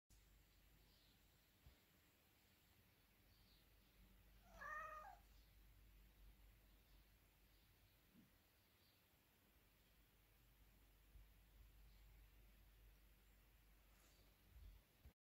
My cat is hungry. 😻 Meine Katze hat Hunger. 😻
How my cat meows, if she is hungry. 😺
Wie meine Katze miaut, wenn sie hungrig ist. 😺